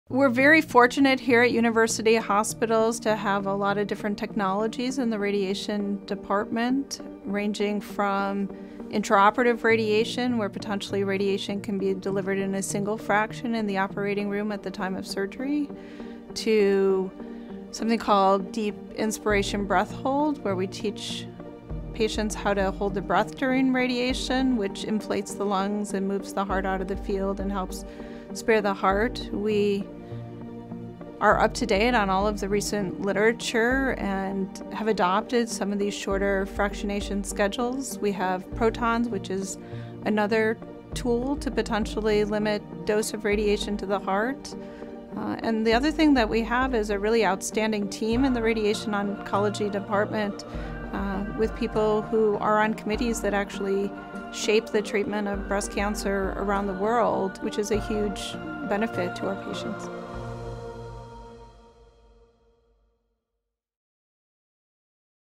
Radiation Therapy Options for Breast Cancer
There are different types of radiation therapy for breast cancer patients.  Janice Lyons, MD, Director of Breast Radiation Oncology at UH Seidman Cancer Center explains the different options including: intraoperative radiation, deep inspiration breath hold, shorter fractionation schedules, and proton therapy.